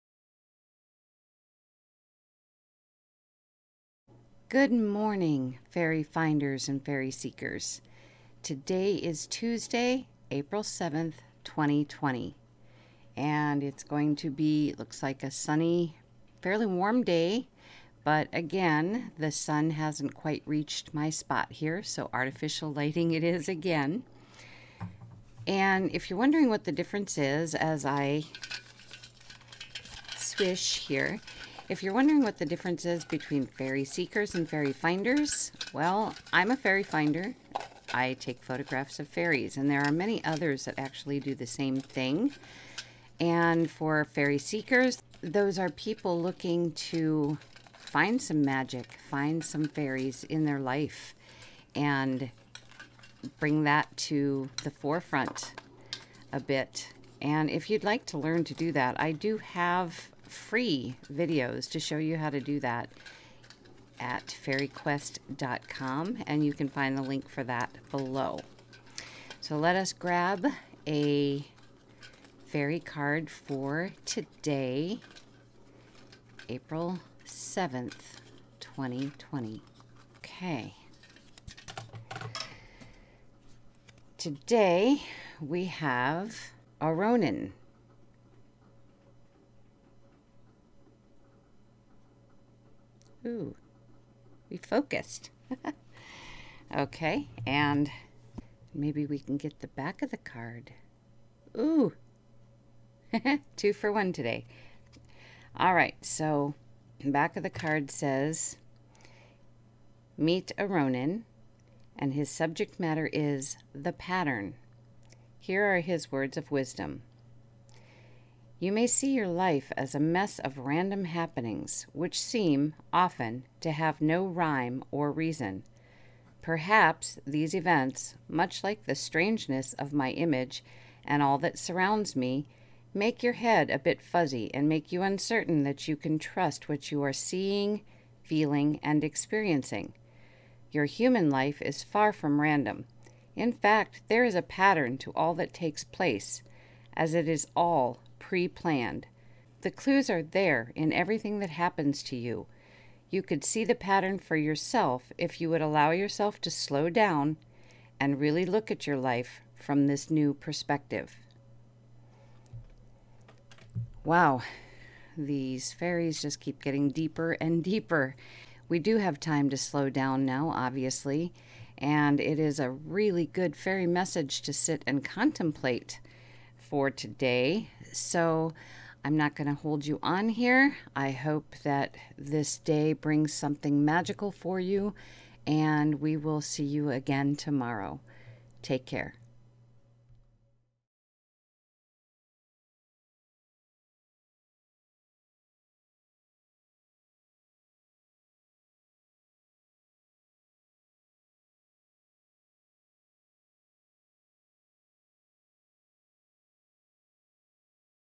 Faerie Message from Auronan/The Pattern/April 7, 2020
Today's message from Auronan is one to consider contemplating. Perhaps what he shares will bring some light as to some questions you may have concerning "why" your life has been as it has been - both from a challenging perspective and an awesome perspective.